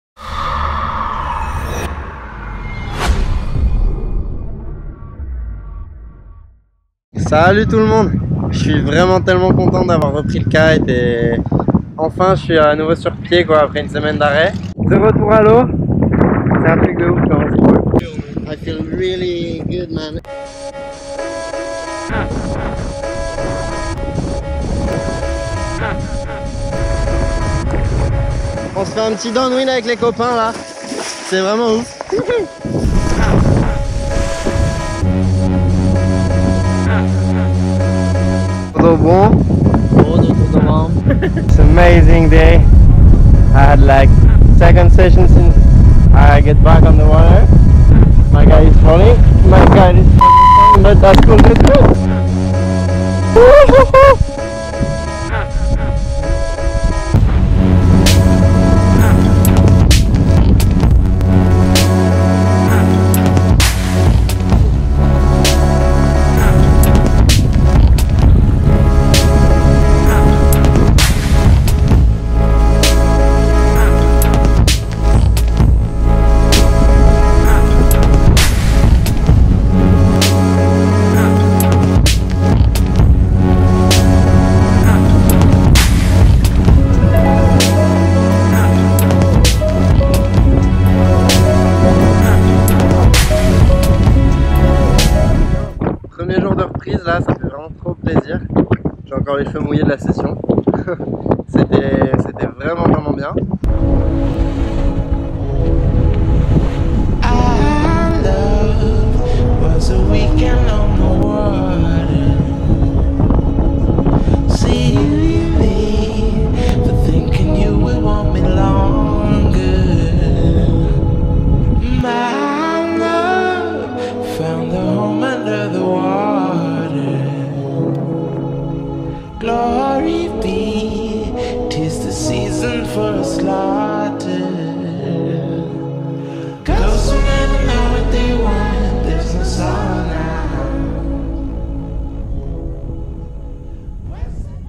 7th week in Brazil - Good to be back
After a while out of the water due to a leg injury, it feels so good to be back on my board again!